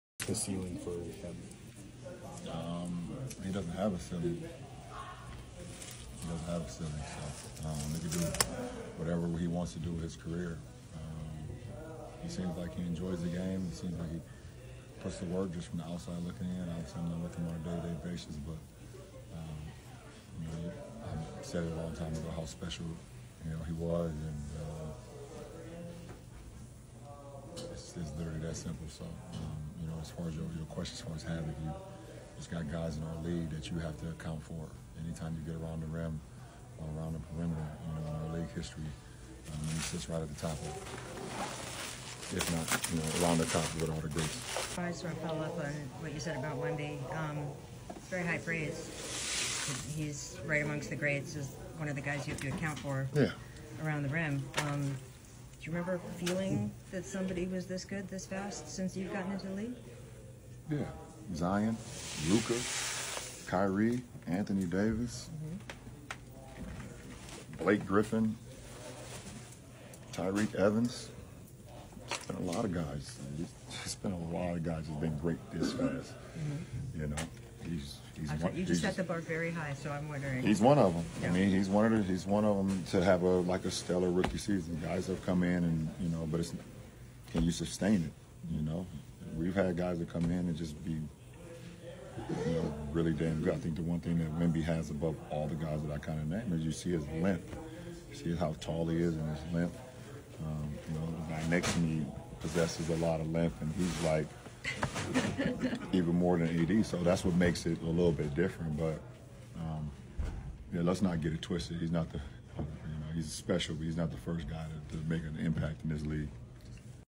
LeBron on Victor Wembanyama: The Next Big Thing in Basketball?
LeBron James, often hailed as the GOAT (Greatest of All Time), continues to astound fans and analysts alike with his unparalleled skill and athleticism in the NBA.